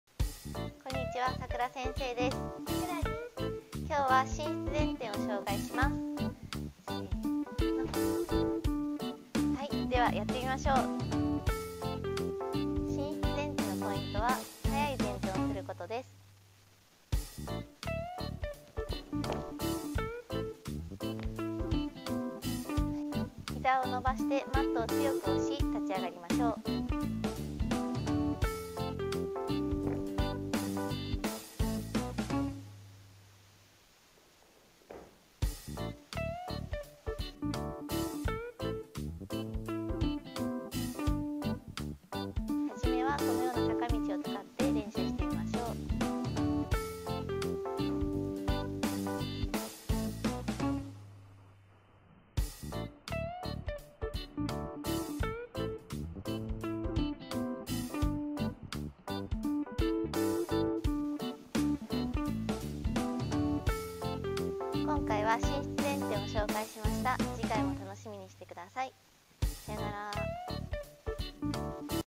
咲くら先生が綺麗な伸膝前転を紹介します‼︎
今回はさくら先生が、さくらちゃんと一緒に綺麗な伸膝前転を紹介します✨

軽く跳んで勢いをつけて、踵が地面に着くタイミングと起き上がるタイミングを合わせて強くマット押します‼︎
このポイントを意識して、膝やつま先まで伸びた伸膝前転を目指して頑張りましょう🔥